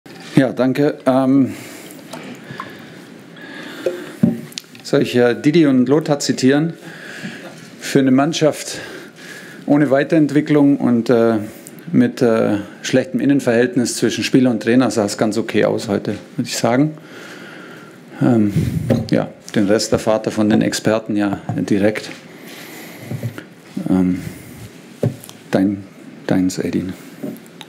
Nach Klassiker: Tuchel-Statement beginnt mit Spitze gegen Hamann & Matthäus
Trotz des 4:0-Erfolgs bei Borussia Dortmund war Bayern-Trainer Thomas Tuchel sichtlich angefressen. Das obligatorische Statement auf der Pressekonferenz nutzt Tuchel für eine klare Kante gegen die Experten Didi Hamann und Lothar Matthäus.